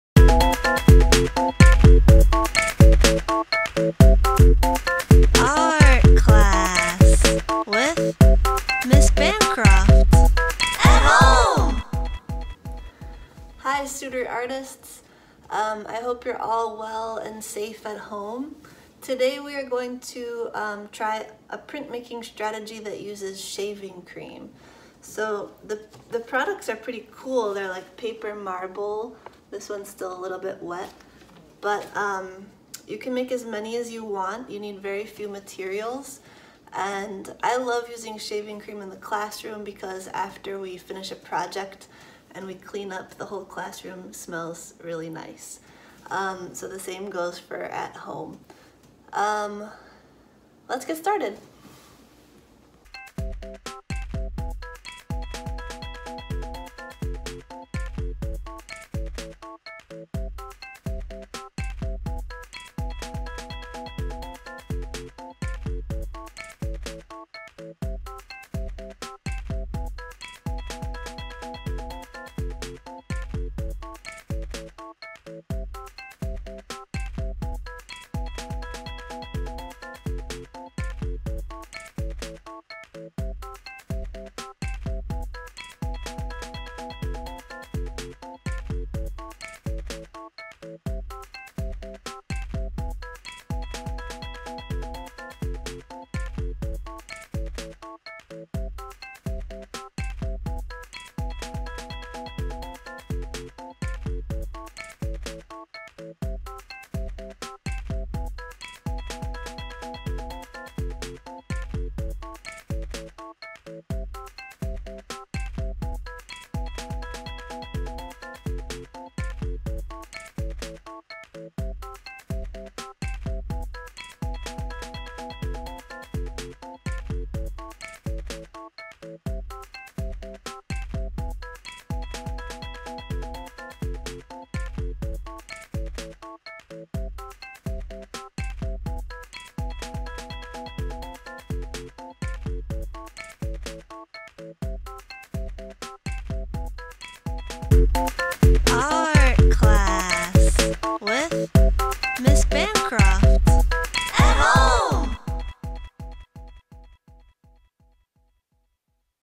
Shaving Cream Prints
Make a (good smelling) mess with me today as we attempt Shaving cream printmaking!